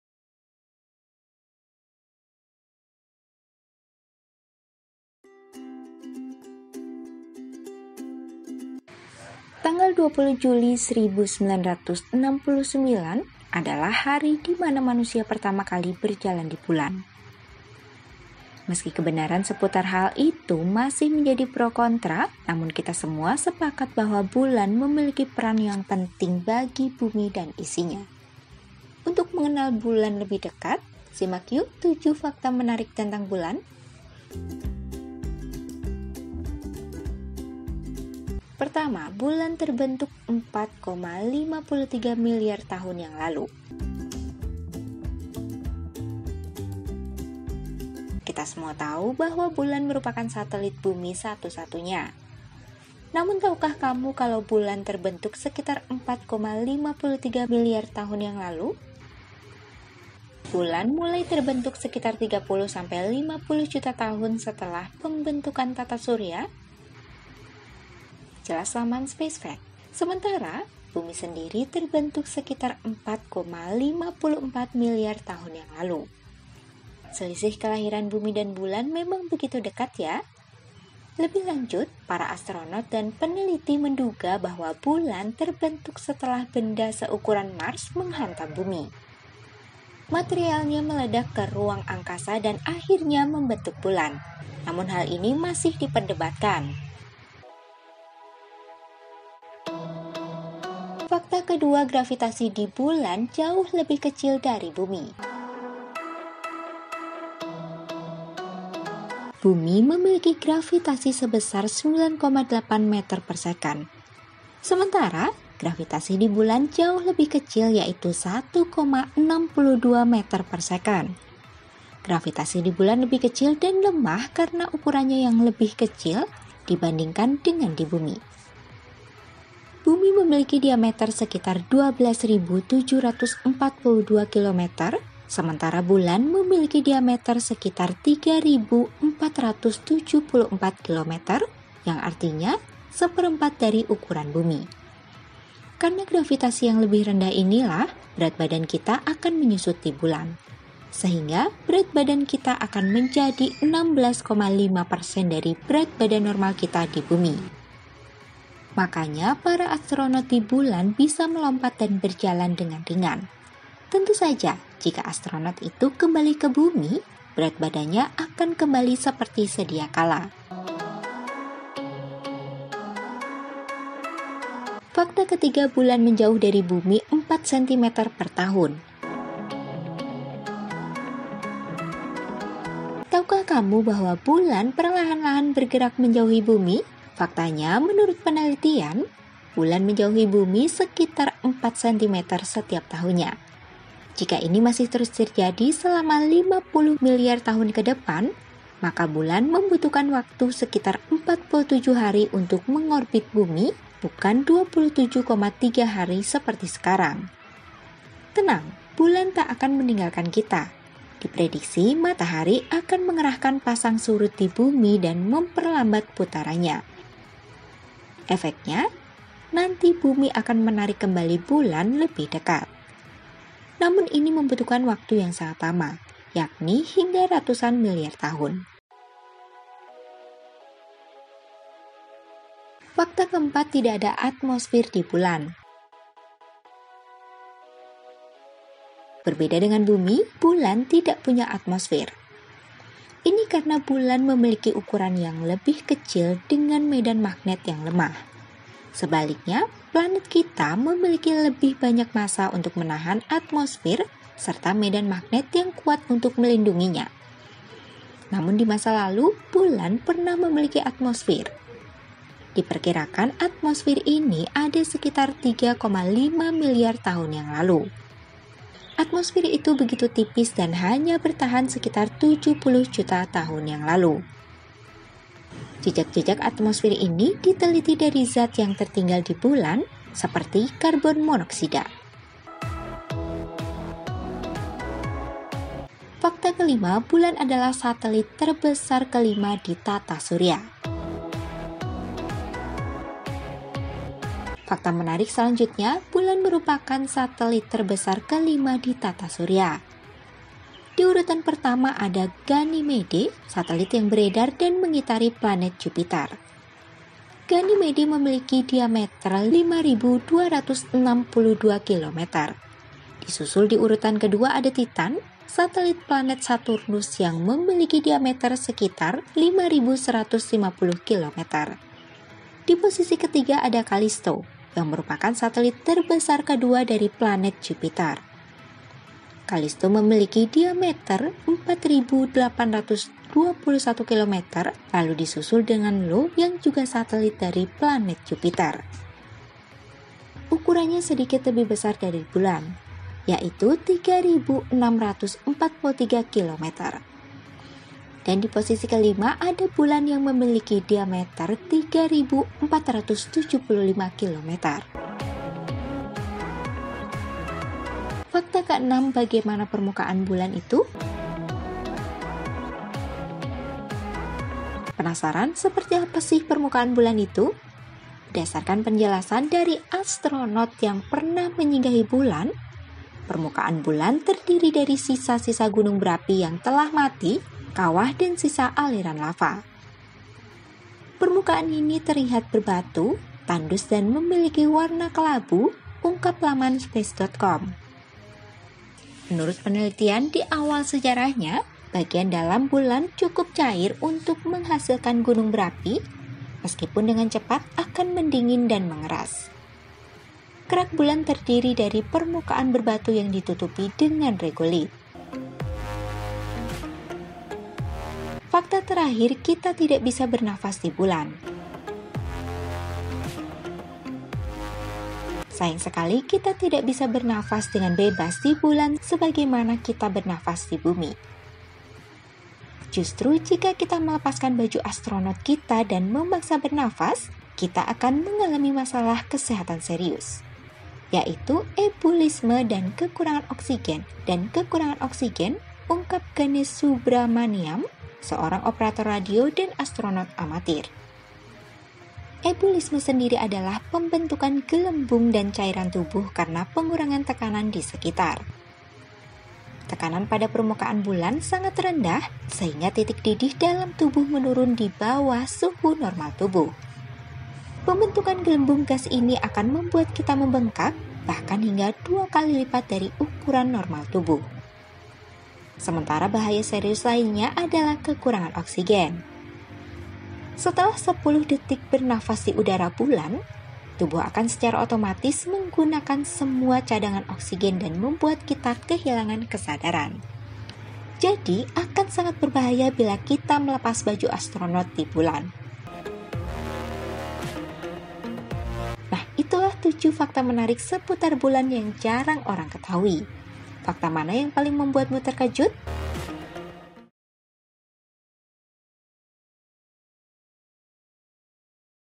7 Fakta Unik Bulan, Benda Langit Yang Setia Mengelilingi Bumi Kita
7 Fakta Unik Bulan, Benda Langit Yang Setia Mengelilingi Bumi Kita

1. Terbentuk 4,53 miliar tahun yang lalu
Kita semua tahu bahwa bulan merupakan satelit bumi satu-satunya. Namun, tahukah kamu kalau bulan terbentuk sekitar 4,53 miliar tahun yang lalu? Bulan mulai terbentuk sekitar 30-50 juta tahun setelah pembentukan tata surya, jelas laman Space Facts. Sementara, bumi sendiri terbentuk sekitar 4,54 miliar tahun yang lalu. Selisih 'kelahiran' bumi dan bulan memang begitu dekat, ya? Lebih lanjut, para astronom dan peneliti menduga bahwa bulan terbentuk setelah benda seukuran Mars menghantam bumi.

2. Gravitasi di bulan jauh lebih kecil dari bumi
Bumi memiliki gravitasi sebesar 9,8m/s². Sementara, gravitasi di bulan jauh lebih kecil, yaitu 1,62m/s². Gravitasi di bulan lebih kecil dan lemah karena ukurannya yang lebih kecil dibandingkan dengan bumi. Bumi memiliki diameter sekitar 12.742 km sementara bulan memiliki diameter sekitar 3.474 km, yang artinya 1/4 dari ukuran bumi. Karena gravitasinya yang lebih rendah inilah berat badan kita akan 'menyusut' di bulan.

3. Bulan menjauh dari bumi 4 cm per tahun
Tahukah kamu bahwa bulan perlahan-lahan bergerak menjauhi bumi? Faktanya, menurut penelitian, bulan menjauhi bumi sekitar 4 cm setiap tahunnya. Jika ini masih terus terjadi selama 50 miliar tahun ke depan, maka bulan membutuhkan waktu sekitar 47 hari untuk mengorbit bumi, bukan 27,3 hari seperti sekarang, tutur laman Space Facts. Tenang, bulan tak akan meninggalkan kita. Diprediksi, matahari akan mengerahkan pasang surut di bumi dan memperlambat putarannya. Efeknya, nanti bumi akan menarik kembali bulan lebih dekat.

4. Tidak ada atmosfer di bulan
Berbeda dengan bumi, bulan tidak punya atmosfer, lho! Ini karena bulan memiliki ukuran yang lebih kecil dengan medan magnet yang lemah. Sebaliknya, planet kita memiliki lebih banyak massa untuk menahan atmosfer serta medan magnet yang kuat untuk melindunginya, ungkap laman Forbes. Namun, di masa lalu bulan pernah memiliki atmosfer, lho! Diperkirakan, atmosfer ini ada sekitar 3,5 miliar tahun yang lalu.

5. Bulan adalah satelit terbesar kelima di tata surya
Fakta menarik selanjutnya, bulan merupakan satelit terbesar kelima di tata surya. Di urutan pertama, ada Ganymede, satelit yang beredar dan mengitari planet Jupiter. Ganymede memiliki diameter 5.262 km. Disusul di urutan kedua ada Titan, satelit planet Saturnus yang memiliki diameter sekitar 5.150 km. Di posisi ketiga ada Callisto, yang merupakan satelit terbesar kedua dari planet Jupiter. Callisto memiliki diameter 4.821 km, lalu disusul dengan Io yang juga satelit dari planet Jupiter.

6. Bagaimana permukaan bulan itu?
Penasaran, seperti apa sih permukaan bulan itu? Berdasarkan penjelasan dari astronom yang pernah menyinggahi bulan, permukaan bulan terdiri dari sisa-sisa gunung berapi yang telah mati, kawah dan sisa aliran lava. Permukaan ini terlihat berbatu, tandus dan memiliki warna kelabu, ungkap laman Space.com. Menurut penelitian, di awal sejarahnya, bagian dalam bulan cukup cair untuk menghasilkan gunung berapi, meskipun dengan cepat akan mendingin dan mengeras. Kerak bulan terdiri dari permukaan berbatu yang ditutupi dengan regolith.

7. Kita tidak bisa bernapas di bulan
Sayang sekali, kita tidak bisa bernapas dengan bebas di bulan sebagaimana kita bernapas di bumi. Justru, jika kita melepaskan baju astronaut kita dan memaksa bernapas, kita akan mengalami masalah kesehatan serius, yaitu ebullisme dan kekurangan oksigen, ungkap Ganesh Subramaniam, seorang operator radio dan astronaut amatir. Ebullisme sendiri adalah pembentukan gelembung dan cairan tubuh karena pengurangan tekanan di sekitar. Tekanan pada permukaan bulan sangat rendah sehingga titik didih dalam tubuh menurun di bawah suhu normal tubuh.

Nah, itulah 7 fakta menarik seputar bulan yang jarang orang ketahui. Fakta mana yang paling membuatmu terkejut?